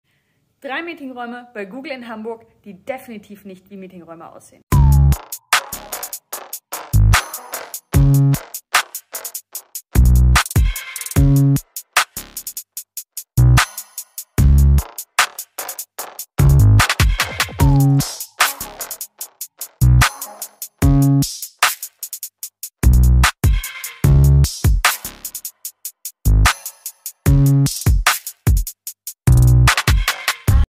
Unglaubliche MEETINGRÄUME bei GOOGLE in HAMBURG
Ja, das sind alles echte Meetingräume im Google-Office in Hamburg